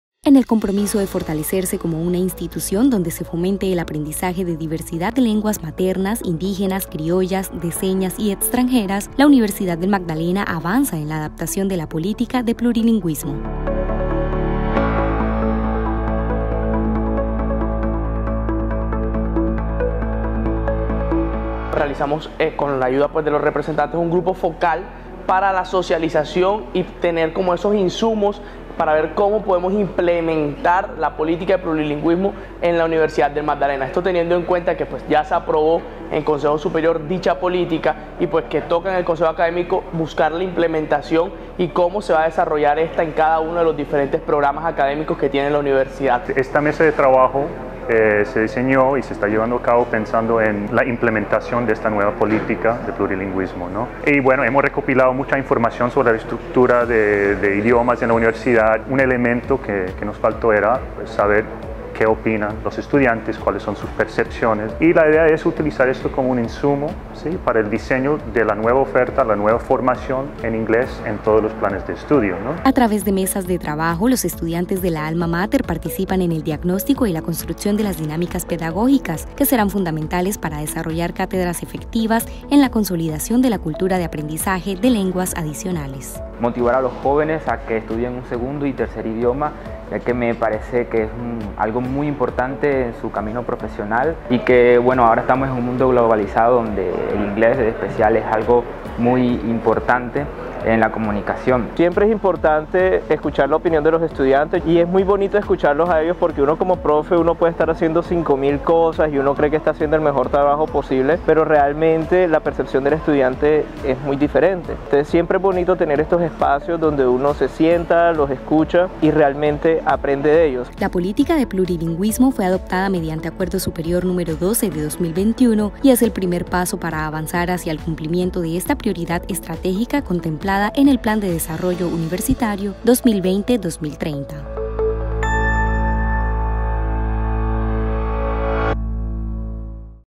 UNIMAGDALENA avanza en la política que impulsa el aprendizaje de lenguas adicionales
Mediante mesas de trabajo, estudiantes de la Universidad del Magdalena participan activamente en el diagnóstico y la construcción de las dinámicas pedagógicas que sentarán la línea base para las cátedras de lenguas maternas, indígenas, criollas, de señas y extranjeras que se fortalecerán con la implementación de la política del plurilingüismo adoptada mediante Acuerdo Superior No. 12 de 2021.